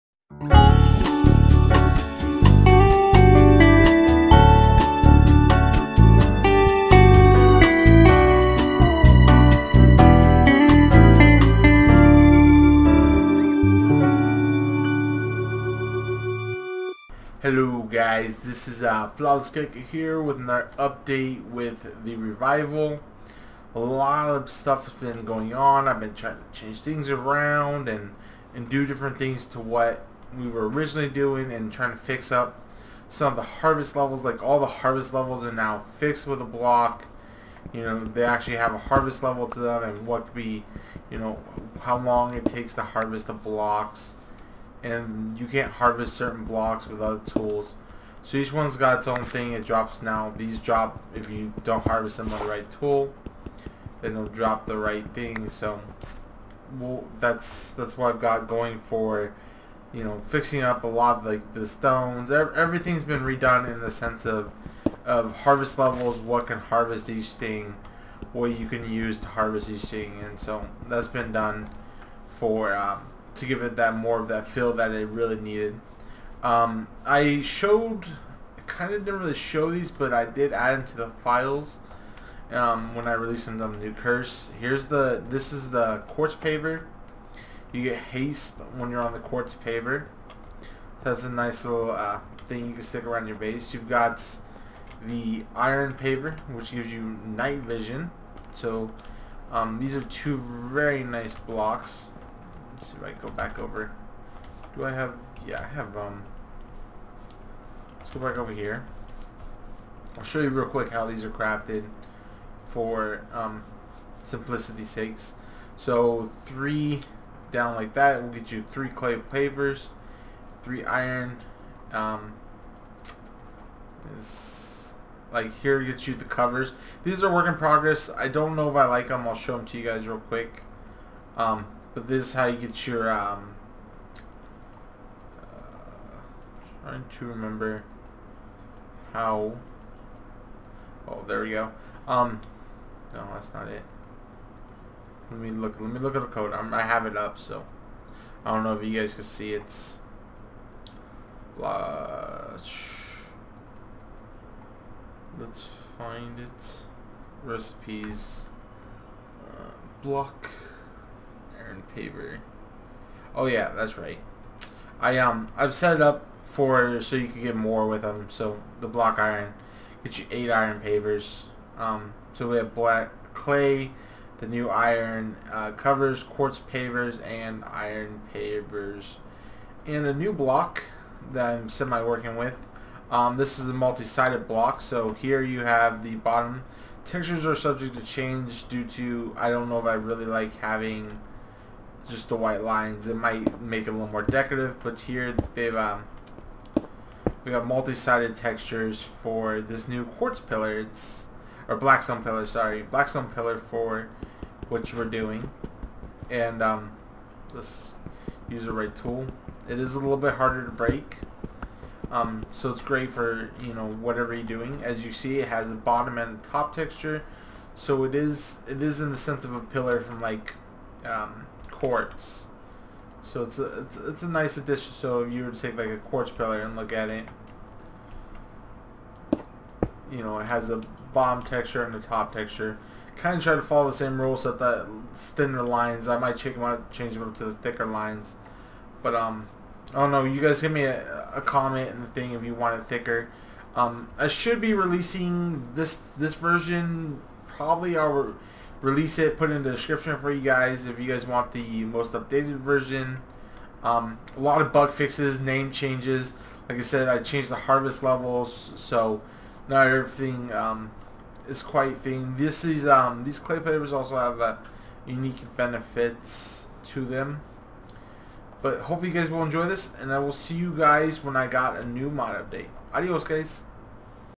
The Revival Mod first Stable release Server side fix
Hi guys and gals the first version of this mod is being released please let me know if there is any bugs.